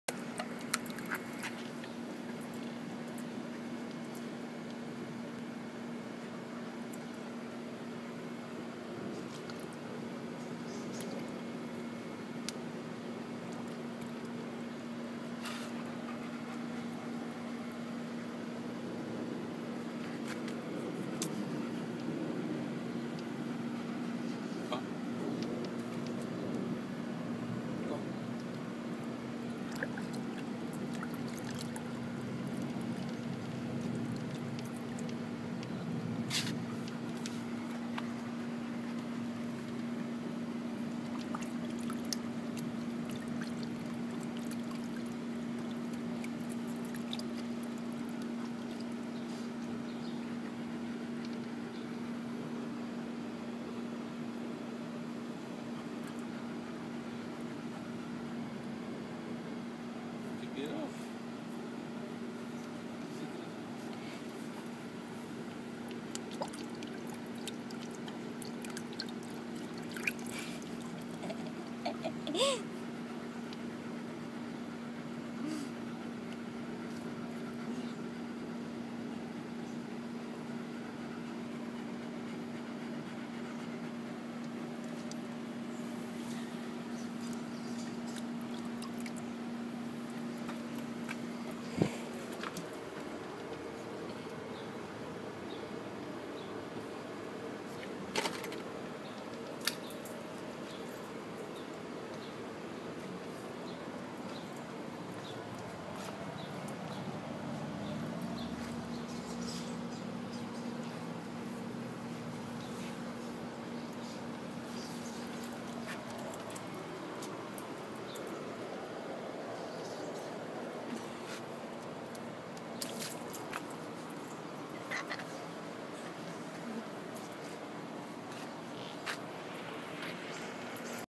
Doberman Evol bobbing for water balloons
Evol was trying to get a water balloon from the bucket but can't so then he got one and pop when he bite it .lol funny